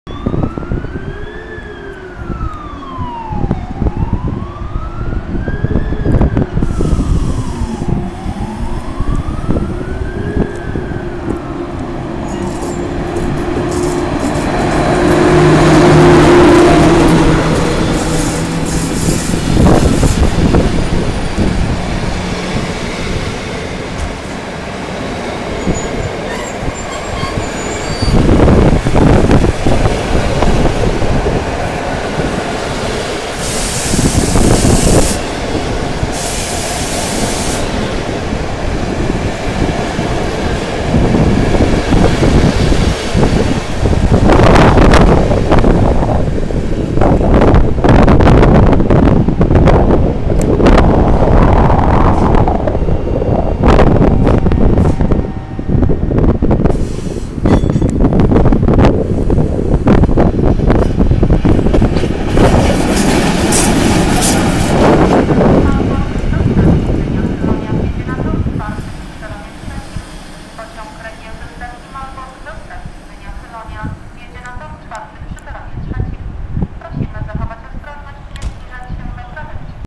EP07 - 1002 zTLK odjeżdźa ze stacji Gdynia Główna
EP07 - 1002 z TLK odjazd z Gdyni Głównej i ładna stonka o nr: SM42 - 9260